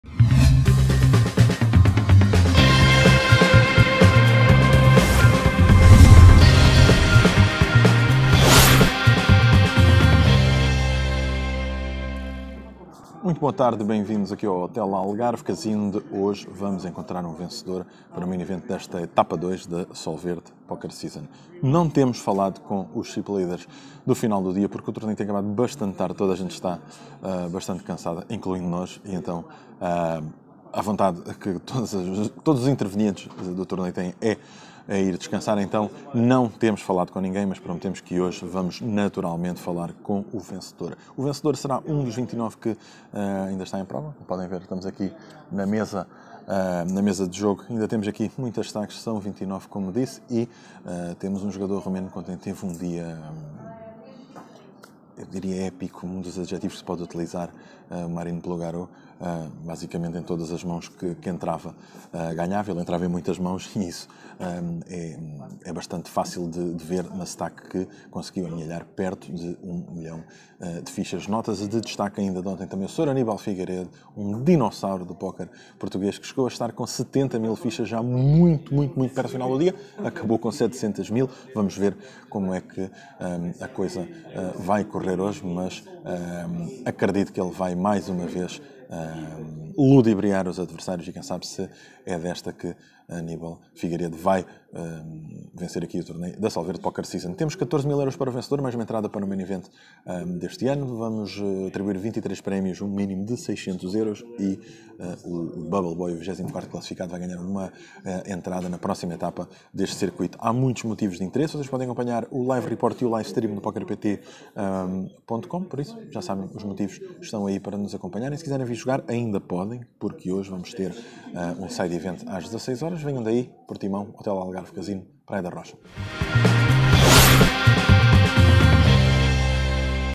Hora de encontrar um campeão para o Main Event da Etapa #2 Solverde Poker Season 2020
Estamos em directo do Hotel Algarve Casino, Portimão, a acompanhar a Etapa #2 Solverde Poker Season 2020